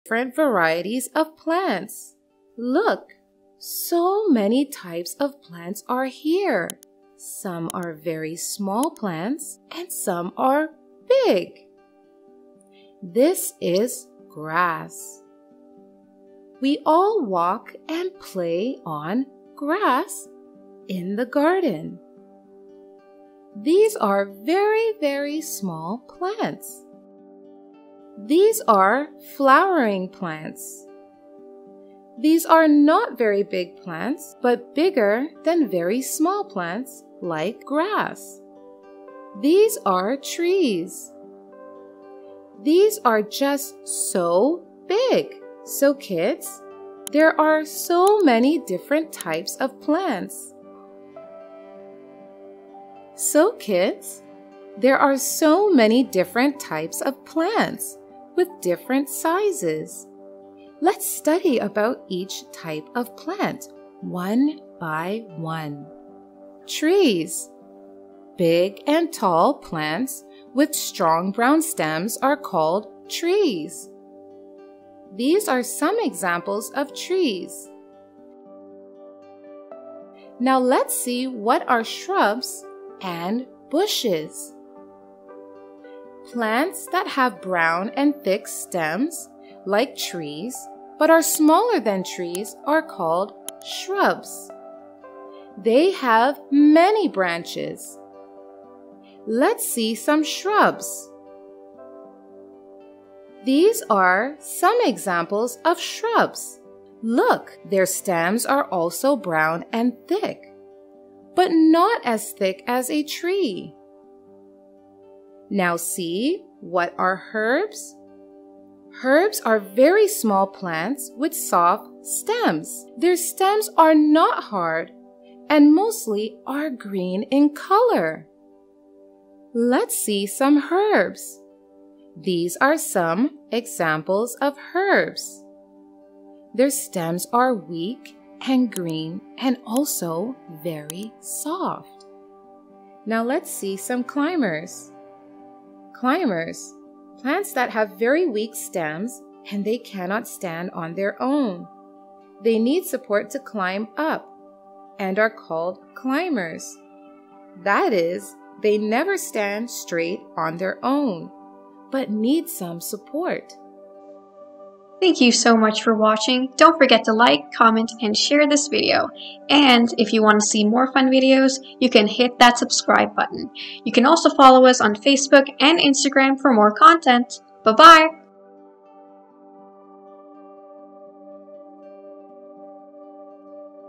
Different Varieties of Plants | Types of Plants
This educational video explains about Plants. This will help them a lot in their daily routine.
Topics Covered:
Plants
Types of plants
Plants around us